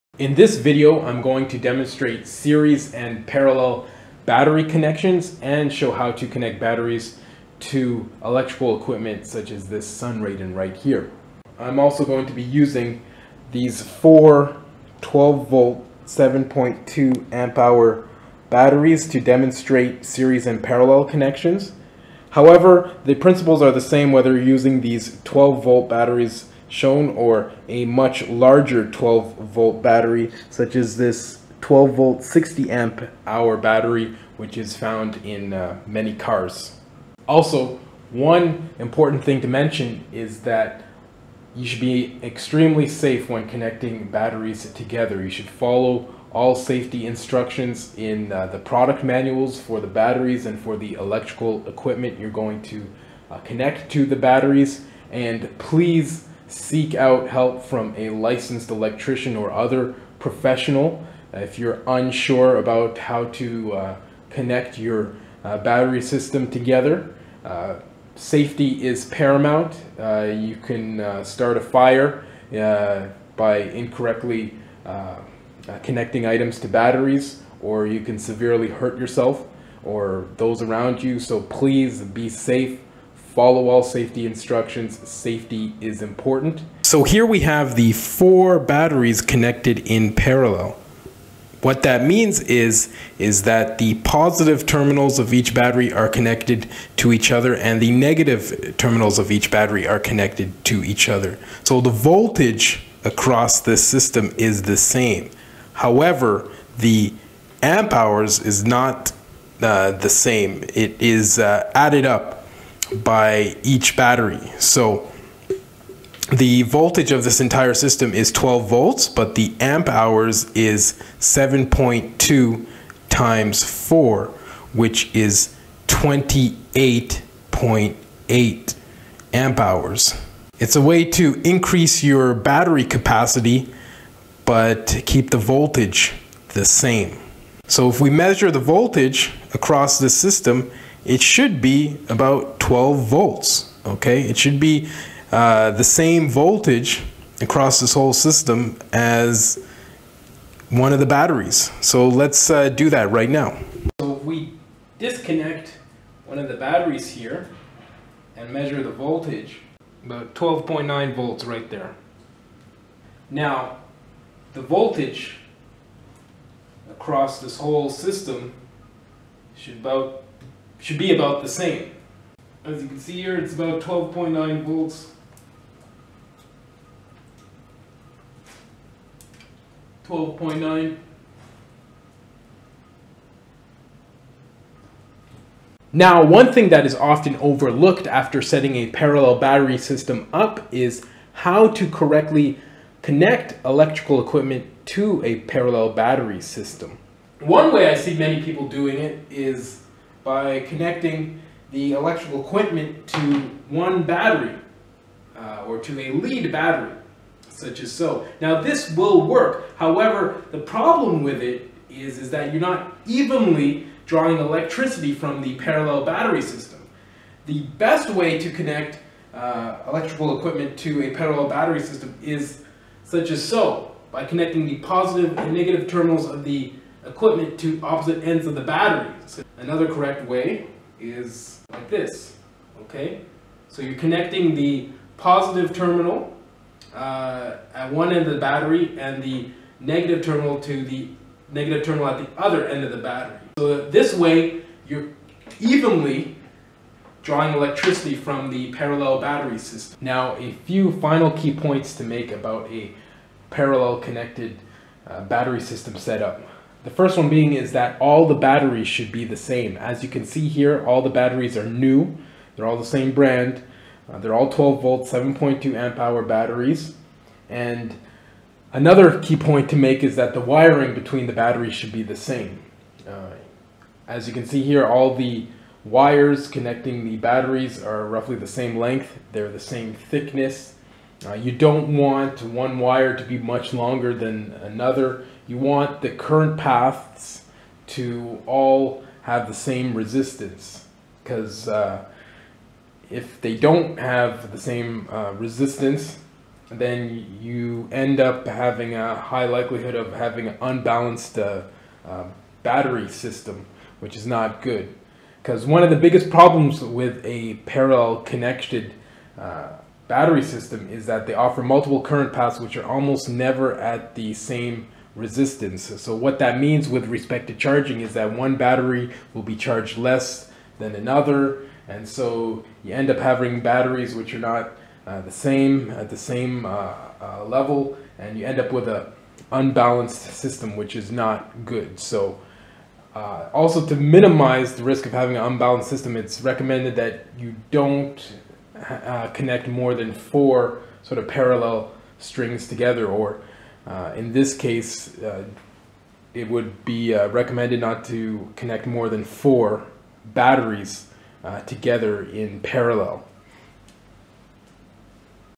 How to Connect Batteries in Series and Parallel - Part 1/2 : Parallel Connections
This first video discusses how to set up a Battery Bank with parallel connections.  Plus, how to properly connect equipment to a parallel battery bank.

The second video discusses Series connections and connecting batteries in both series and parallel.

This video is for demonstration purposes only and not to be used as a definitive guide for those who are unfamiliar and inexperienced with connecting batteries.

 Please seek assistance from an electrician or other appropriate licensed professional before connecting your batteries.  Improperly connecting equipment to  batteries is hazardous, can cause fires and may result in injury.